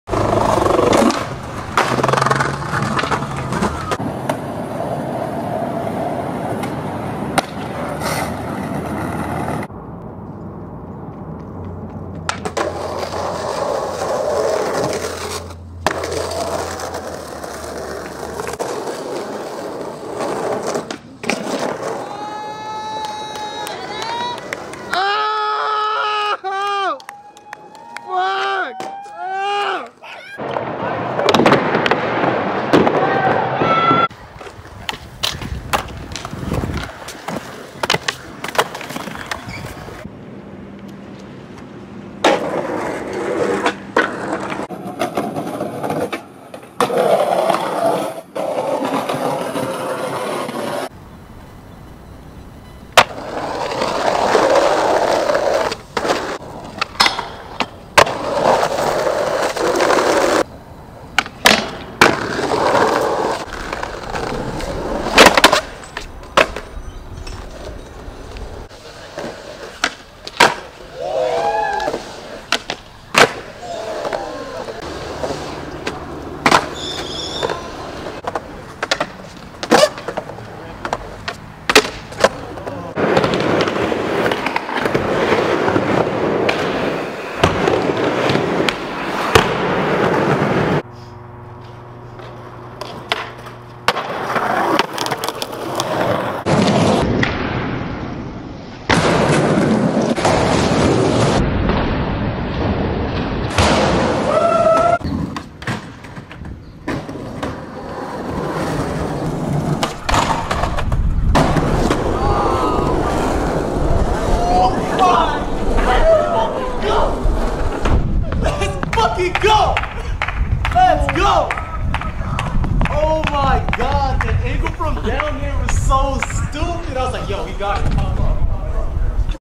CHRIS HIETT | TRUE SKATEBOARD MAG MEDIA | EXCLUSIVE VIDEO
As seen on TSM 'LIVE SHOW' S3 ep2, Chris Hiett was interviewed for future Sk8'r section & we dropped a exclusive video part too

Amazon Music